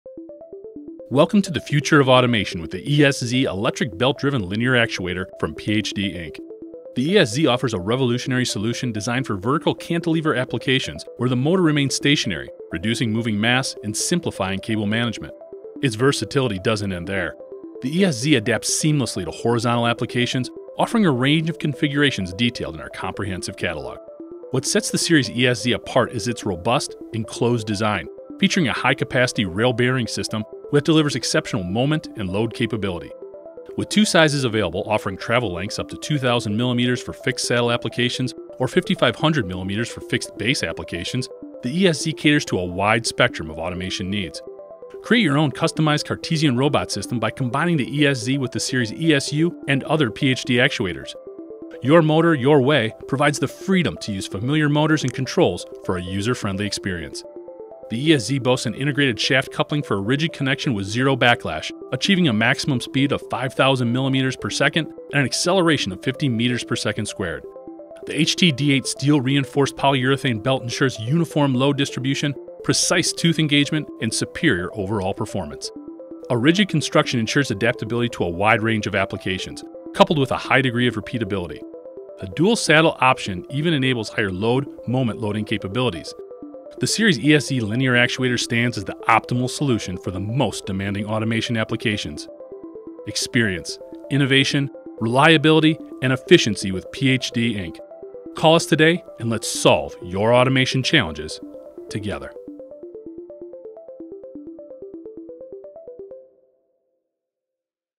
Series ESZ Electric Belt-Driven Linear Cantilever Actuators
Series ESZ Electric Belt-Driven Linear Cantilever Actuators feature a robust, enclosed design with a high-capacity rail bearing system delivering unparalleled speed, thrust, and precision. Available in two sizes, with travel lengths up to 5,500 mm and speeds to 5,000 mm/s, these electric linear actuators can be combined with the Series ESU and other PHD actuators to create virtually any Cartesian system to meet your robotic needs. The choice of saddle combinations, single, dual, and independently driven dual saddles, makes it ideal for a multitude of vertical and horizontal applications. Other key features are an integrated shaft coupling(s) for a rigid connection and zero backlash, HTD8 steel reinforced drive belt for superior performance, easy access belt tensioning system, and Your Motor, Your Way allows your choice of motor and controls flexibility.